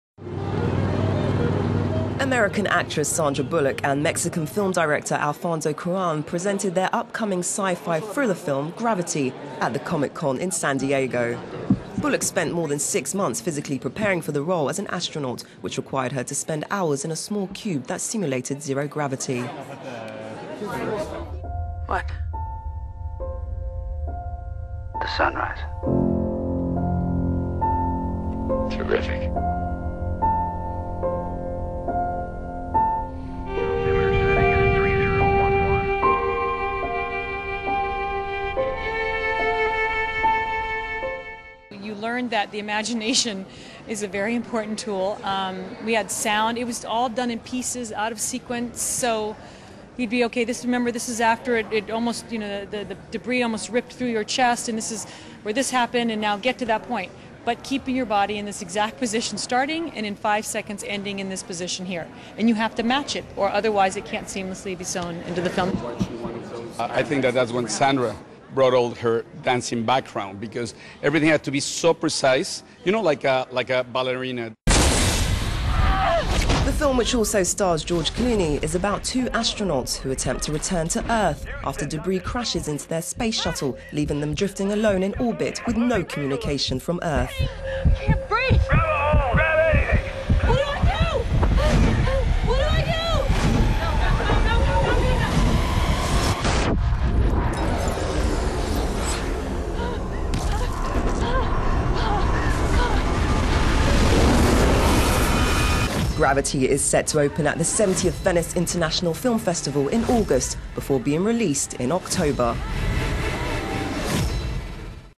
Super-fit Bullock feels the gravity of being lost in space - cinema
American actress Sandra Bullock and Mexican film director Alfonso Cuarón have presented their...

American actress Sandra Bullock and Mexican film director Alfonso Cuarón have presented their upcoming thriller film 'Gravity' at Comic-Con in San Diego.

Bullock spent more than six months preparing physically for the role as an astronaut, which required her to spend hours in a small cube that simulated zero gravity.

She said she had to be really creative: "You learn that the imagination is a very important tool. We had sound. It was all done in pieces, out of sequence. So they'd be like, 'OK, this is when the debris almost ripped through your chest, and this is where this happened and now get to that point, but keeping your body in this exact position starting and in five seconds ending in this position here'. And you have to match it otherwise it can't seamlessly be sown into the film."

Director Alfonso Cuaron praised her athleticism in the role: "I think that's when Sandra brought all her dancing background. Everything had to be so precise, you know like a ballerina."

The film, which also stars George Clooney, is about two astronauts who attempt to return to Earth after debris hits their space shuttle, leaving them drifting alone in orbit - with no communication from Earth.

Although the trailer has audible explosions in it, the director has confirmed that scenes in space will be silent as there is no sound in space.